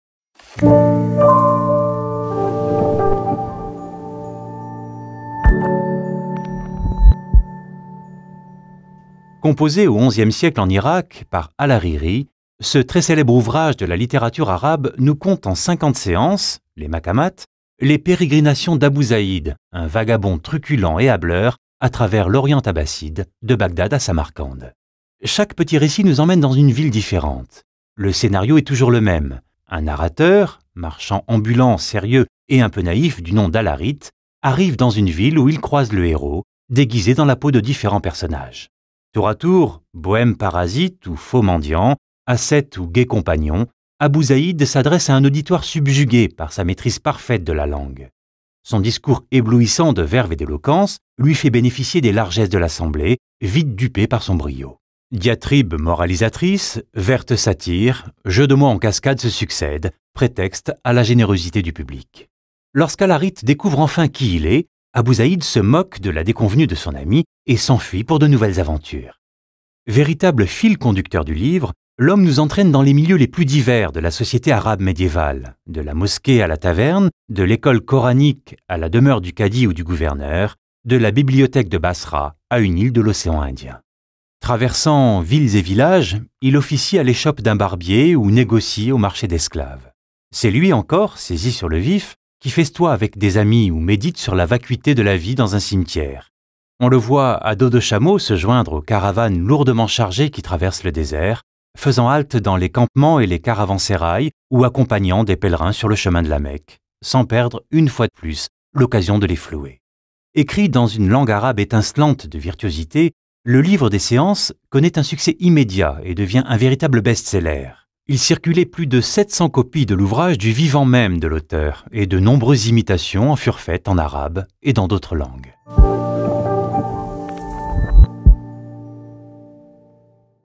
Le Livre des Séances
Composé au XIe siècle en Irak par al-Harîrî, ce très célèbre ouvrage de la littérature arabe, nous conte en cinquante séances (Les Maqâmâts), les pérégrinations d'Abû Zayd, un vagabond truculent et hâbleur, à travers l'Orient abbasside, de Bagdad à Samarcande. Chaque petit récit nous emmène dans une ville différente. Le scénario est toujours le même. Un narrateur, marchand ambulant, sérieux et un peu naïf, du nom d'al-Hârith, arrive dans une ville où il croise le héros, déguisé dans la peau de différents personnages…

Découvrir le dossier "La peinture arabe" sur les Essentiels de la BnF